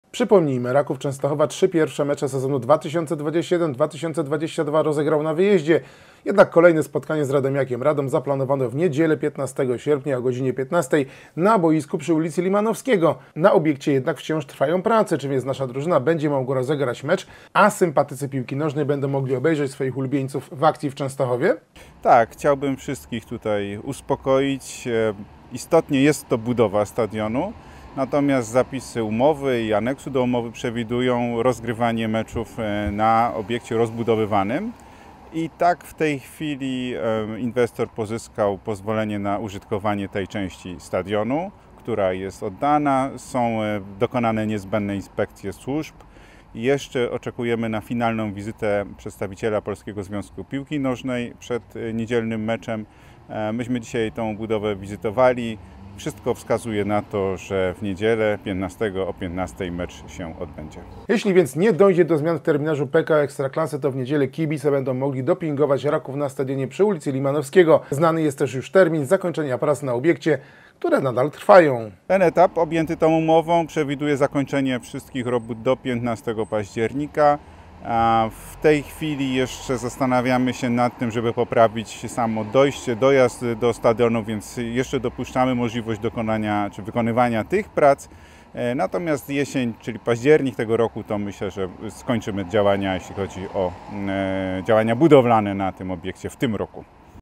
Raków na Limanowskiego?
Czy w niedzielę 15 sierpnia piłkarze Rakowa Częstochowa zagrają na stadionie przy ulicy Limanowskiego? To sprawdzamy w naszym materiale.